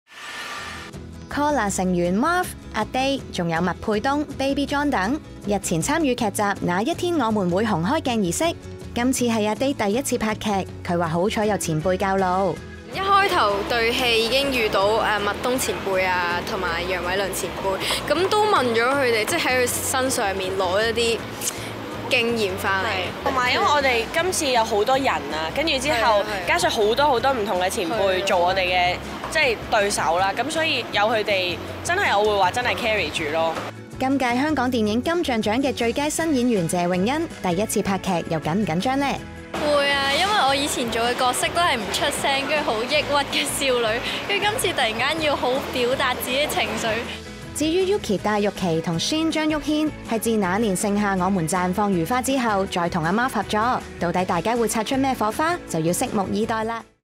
📢又有新劇《哪一天我們會紅》正式開鏡📹 火花滿滿🔥
好消息陸續有嚟📣 我哋唔好俾佢停‼️
📢又有新劇開鏡喇🤩📢《哪一天我們會紅》
參與劇集嘅演員陣容鼎盛🔥演藝界前輩➕新晉演員拍住上🤝
舊拍檔➕新合作😝 真係好令人期待！